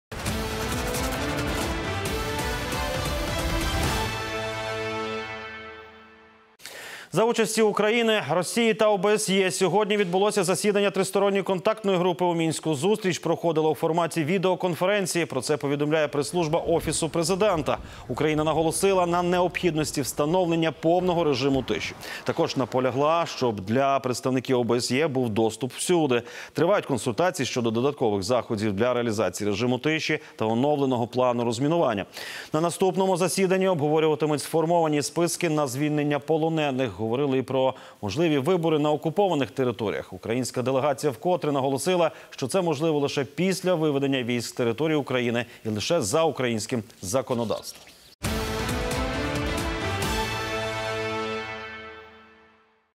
Заседание ТКГ прошло в формате видеоконференции с участием Украины, РФ и ОБСЕ
При участии Украины, России и ОБСЕ сегодня состоялось заседание Трехсторонней контактной группы в Минске.